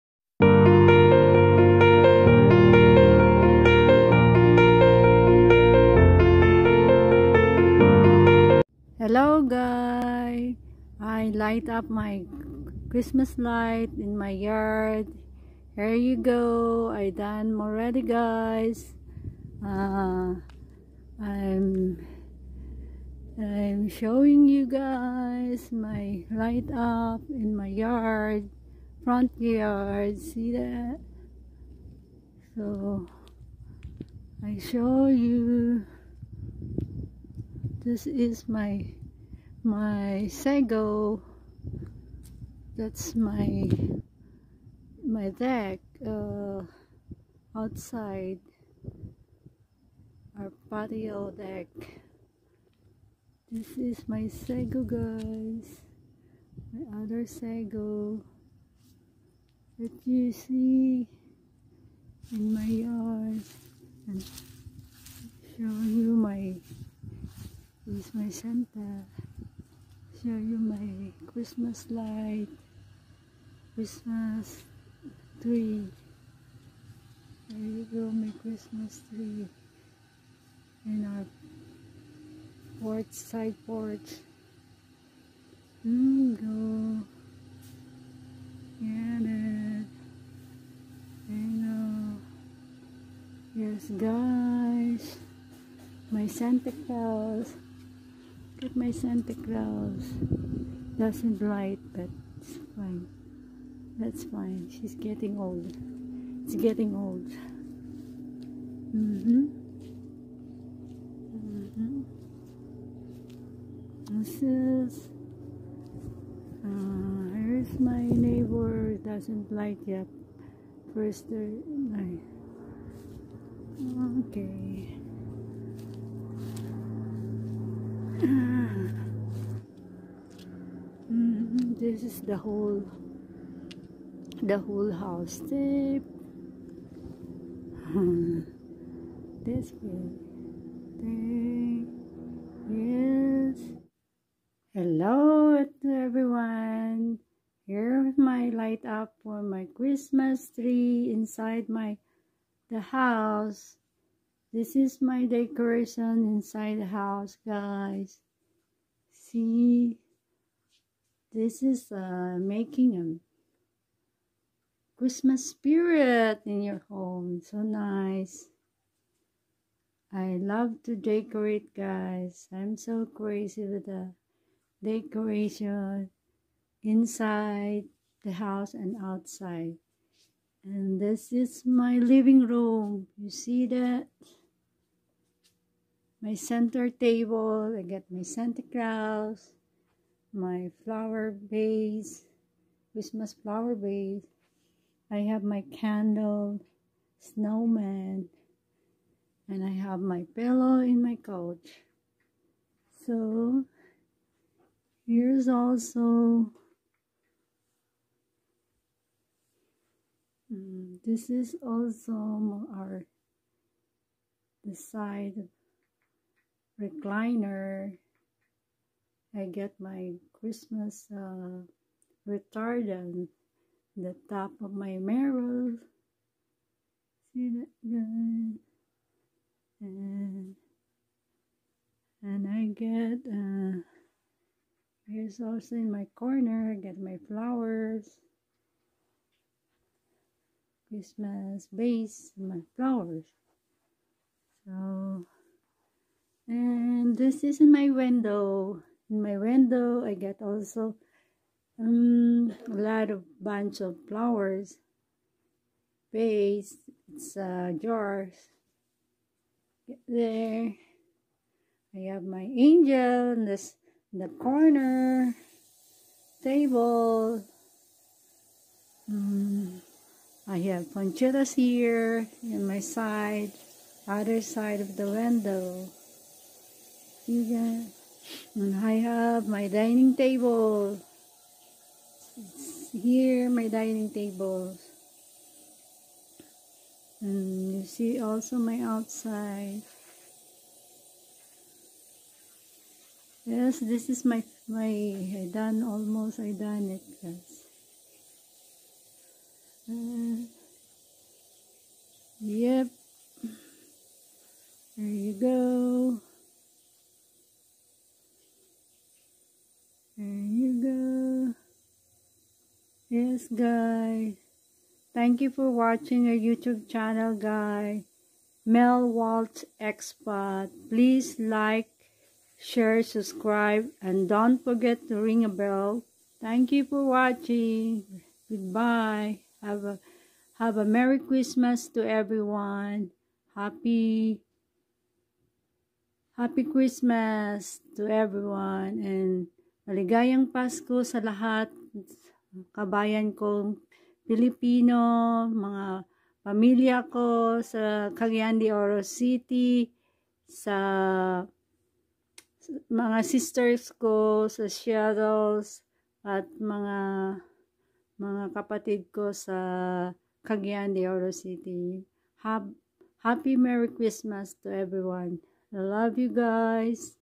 MelWalt Expats Say Merry Christmas House Lights up Outdoors and indoors our Christmas Tradition.
HOLIDAY LIGHTS:Christmas Traditional